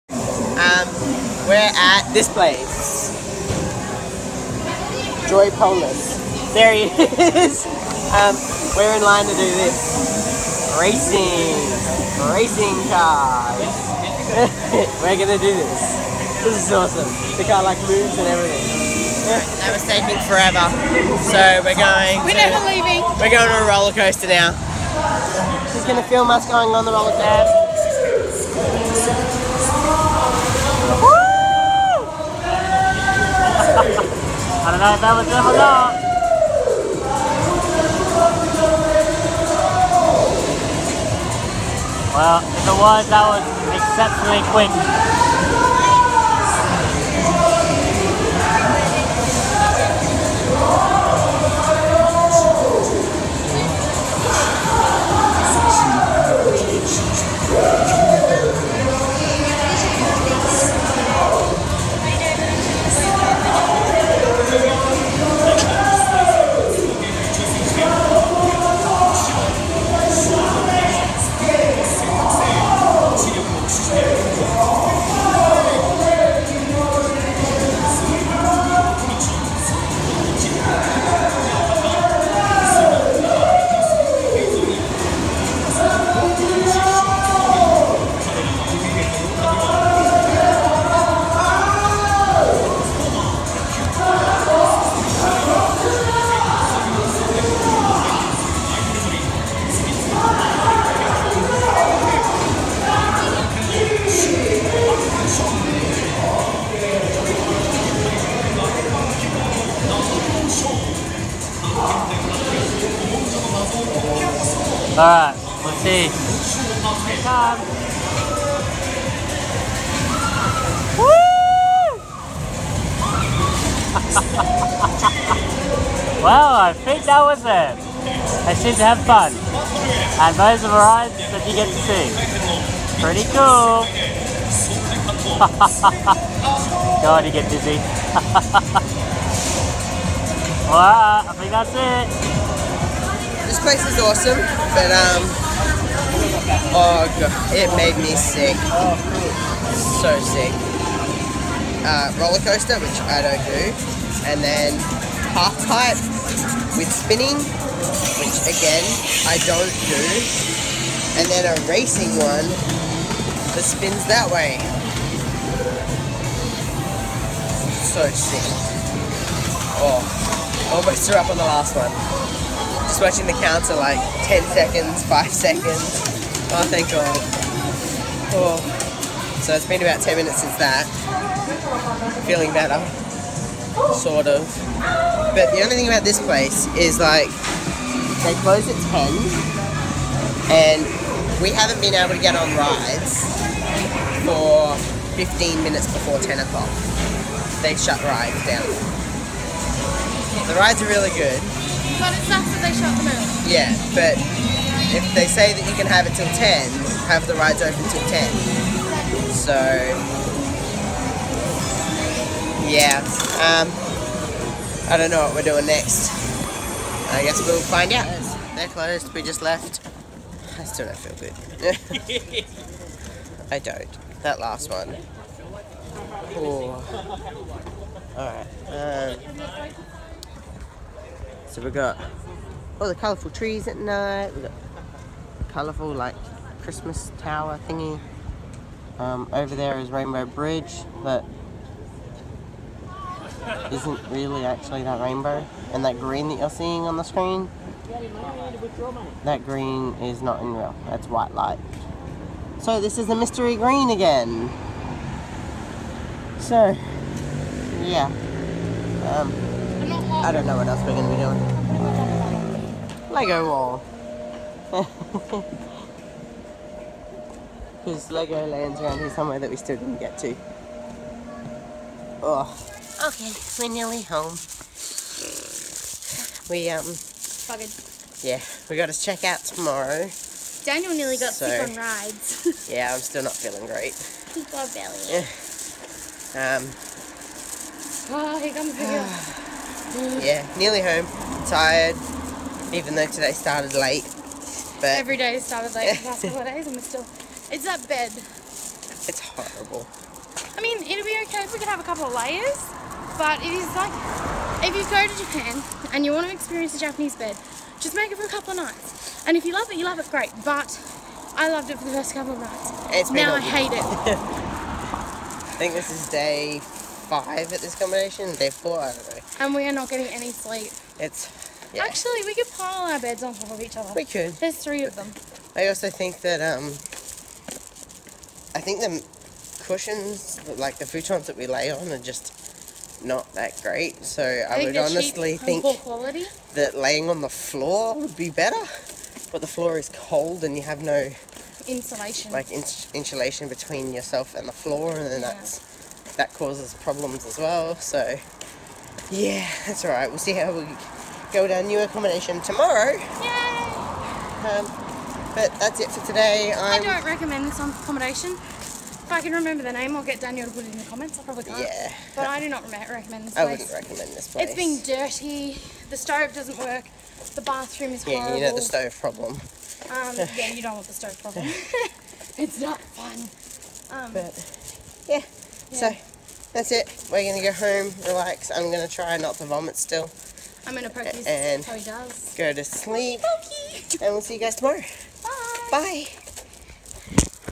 Japan Day 18 Part 2 - Awesome Amusement Park and Pale Face
Visiting an amusement park inside a shopping centre and someone gets sick.